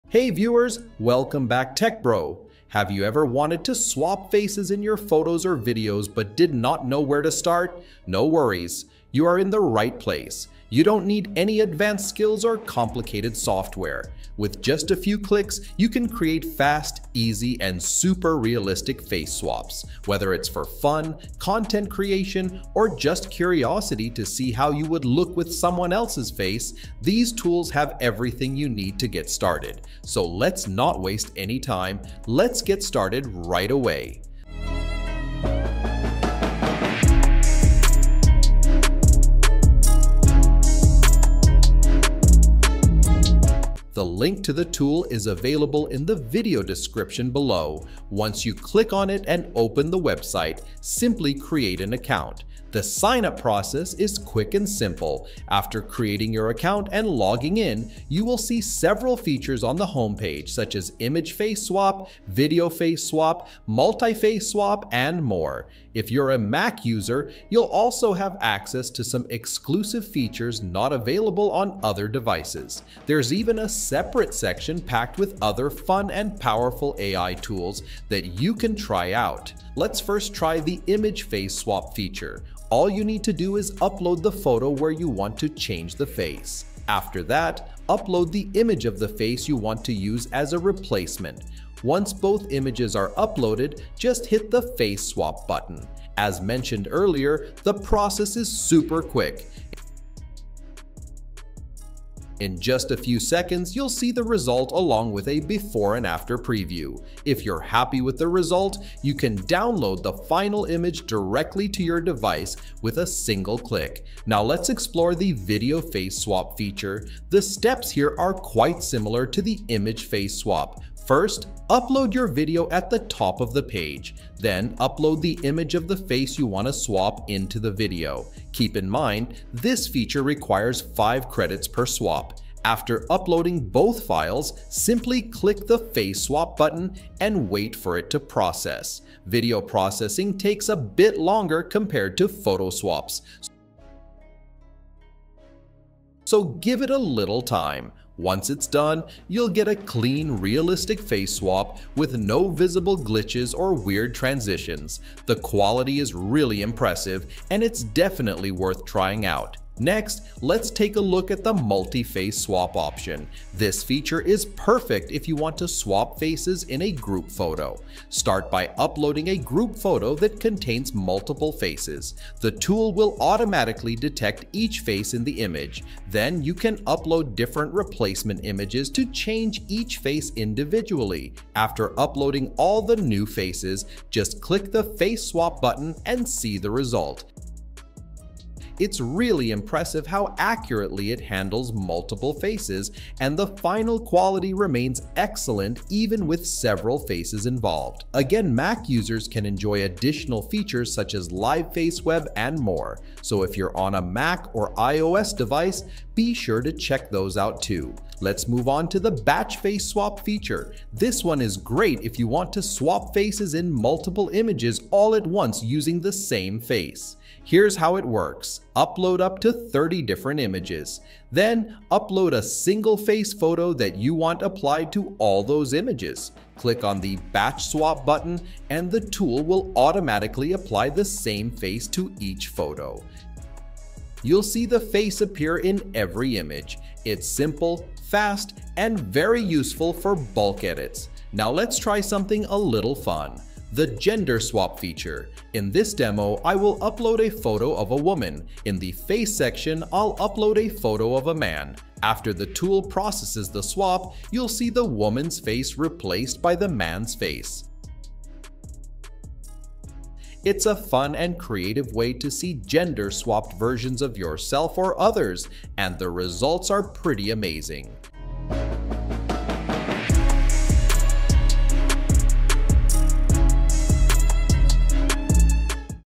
Best Free Face Swap Tool 2025 | Photo & Video Face Swap Tutorial (No Skills Needed!)
In this video, I’ll show you how to easily swap faces in your photos and videos using one of the best online AI face swap tools out there. Whether you’re just having fun, creating content, or curious to see how you'd look with someone else’s face — this tool does it all with just a few clicks!